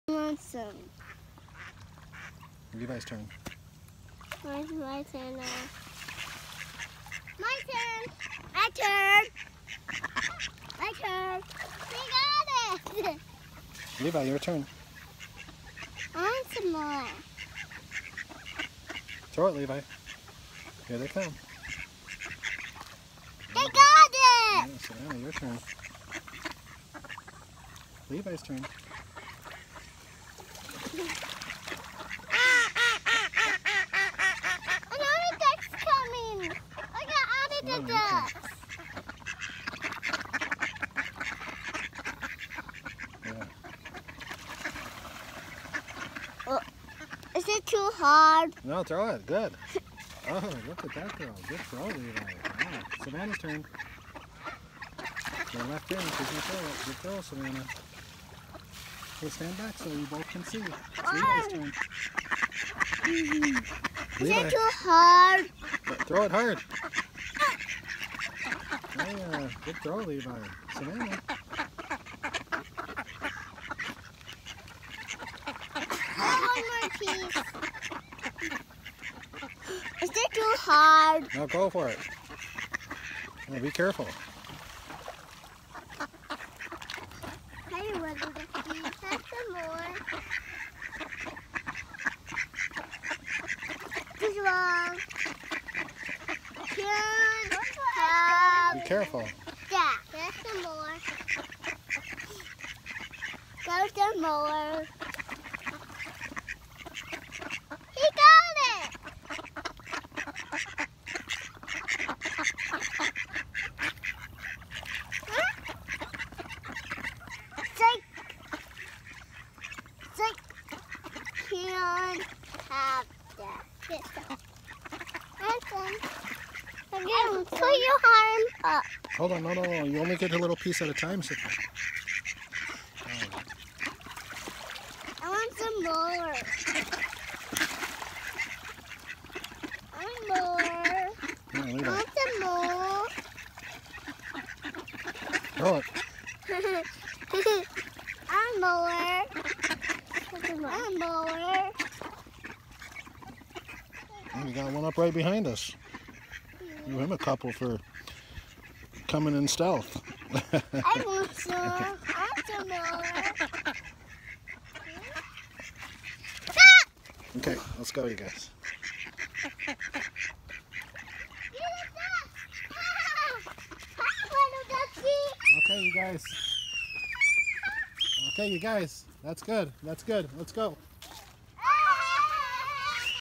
Feeding the ducks at Brittany park
Levi and Savannah and Charlotte